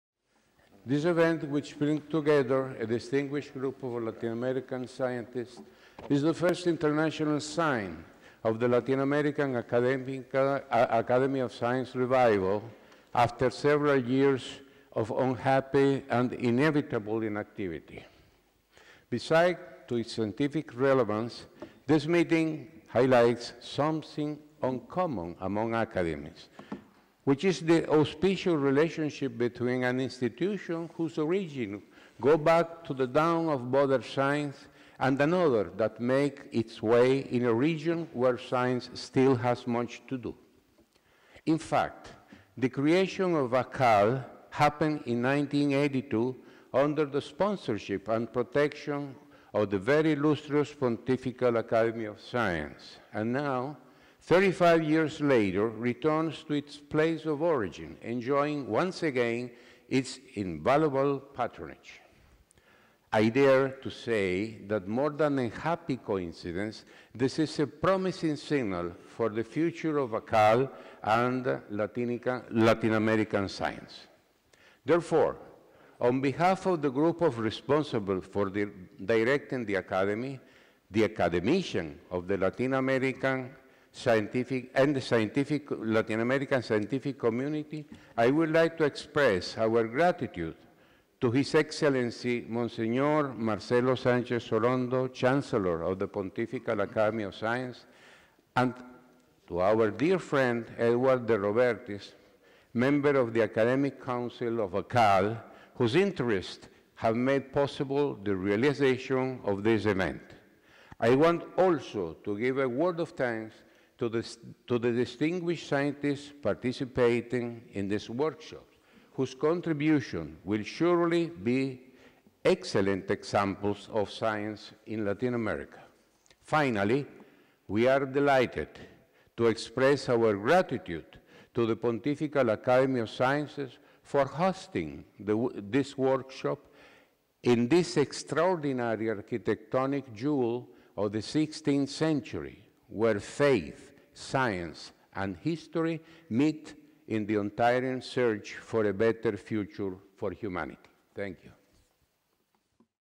1. Word of Thanks | Claudio Bifano
Workshop 23-24 October 2017

The PAS Workshop on Cell Biology and Genetics will bring together a group of scientists from the Pontifical Academy and the Academia de Ciencias de América Latina (ACAL). The main goal will be to report on recent discoveries in Cell Biology.  It will also include discussion on how to overcome the main challenges for scientific research in Latin America.  Latin America is a vast continent that has experienced significant improvements in scientific capabilities in the past decades. There have also been setbacks and structural challenges. Science is a driving force for development with many social benefits, and scientific advances in many areas are making a great impact worldwide. Among them is cell biology which will be the focus of this workshop.  Scientific sessions will bring together experts in the fields of membrane biology, genetics, cell signaling, neurobiology and biomedical applications of cell biology.